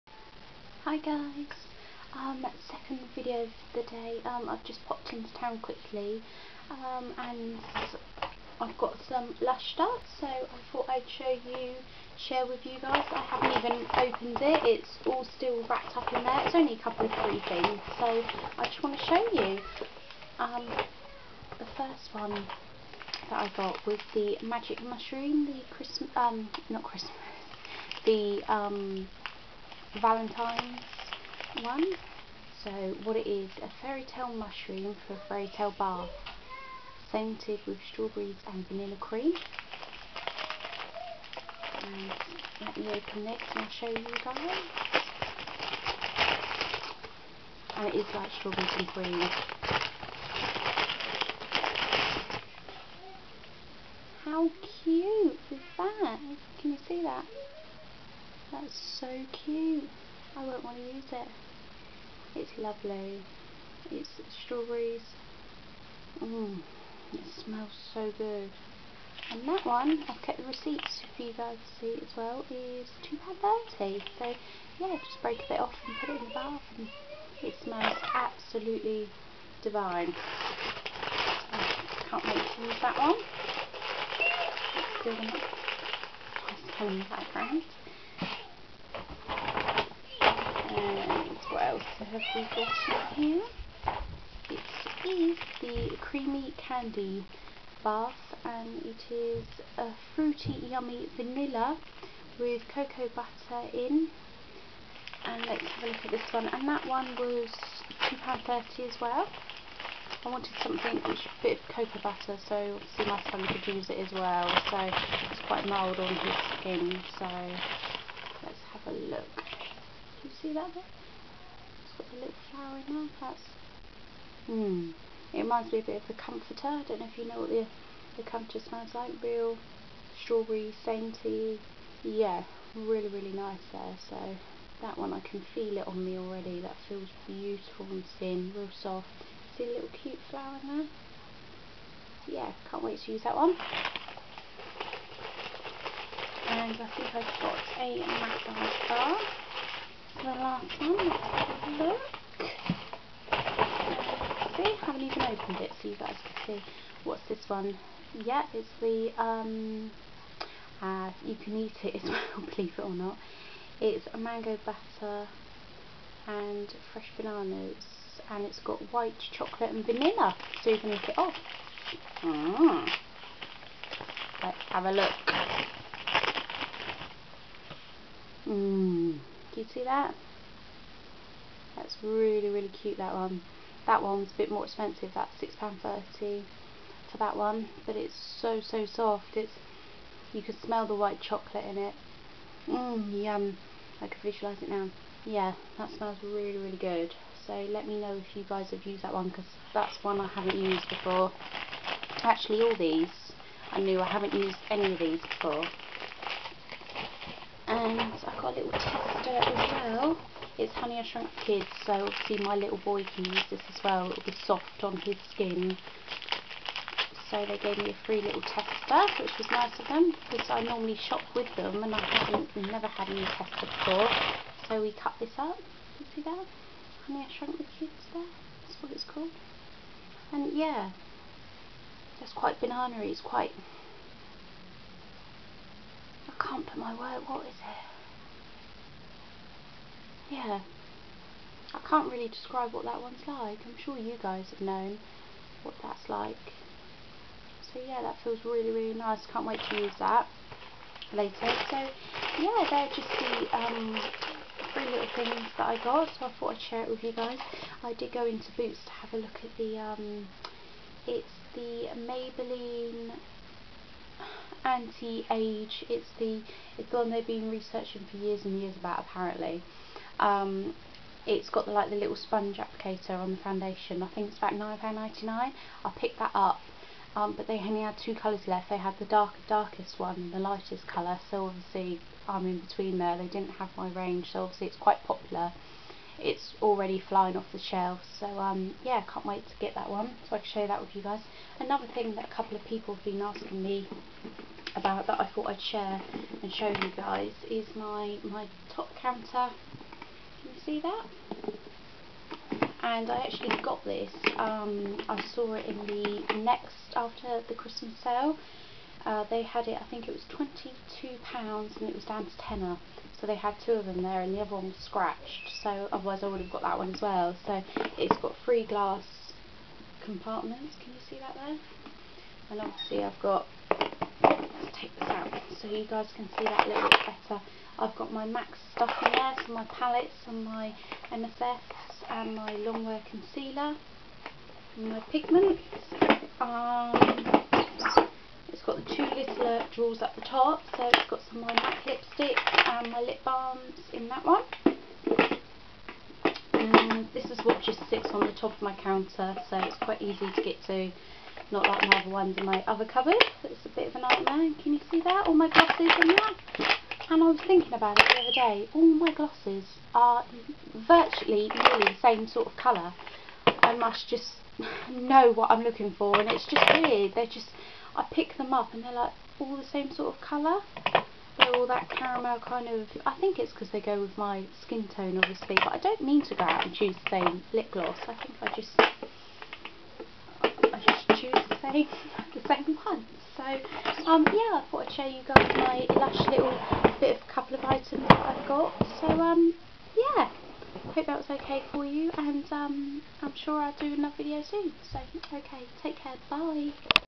Little Lush Haul
Magic Mushroom
Honey I washed the kids
Creamy Dreamy Bath Melt
Body Message Bar White Chocolate & bananas
NEXT glass unit. I use for Make up Storage
was £22 but got htis in Christmas Sale for £10.00